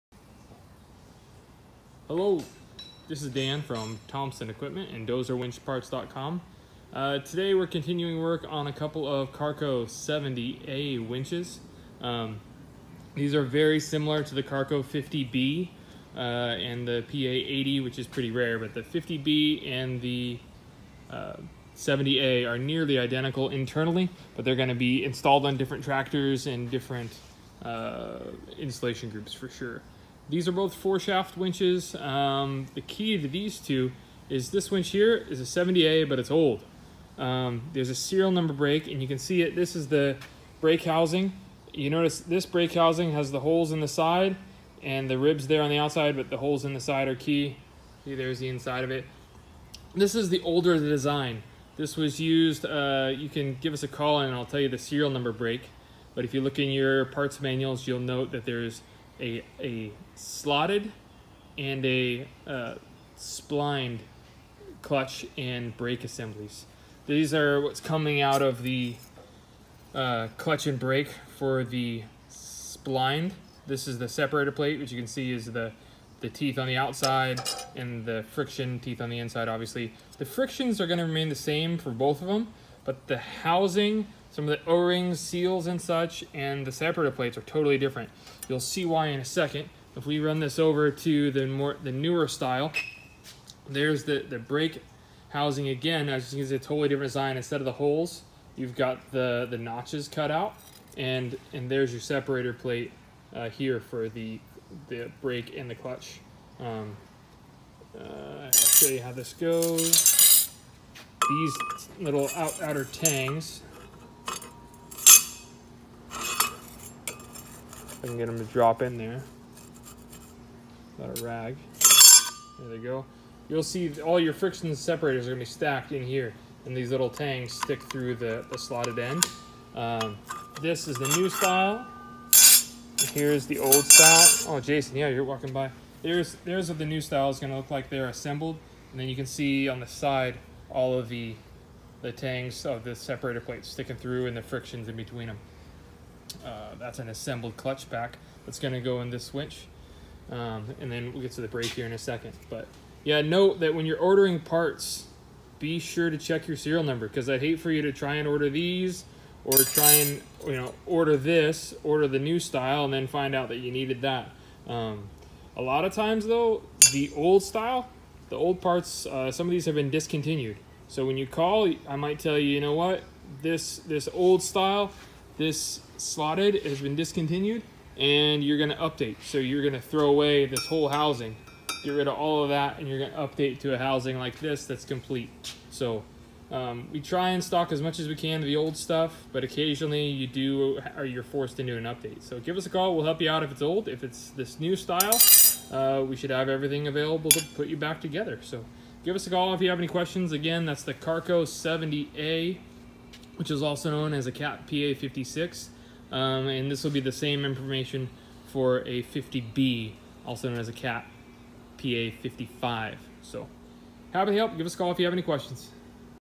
Carco PTO clutch changes
The Carco 50b and 70a (PA55 & PA56) have been in production a long time. There are significant changes to the clutch and brake that are very important when ordering parts.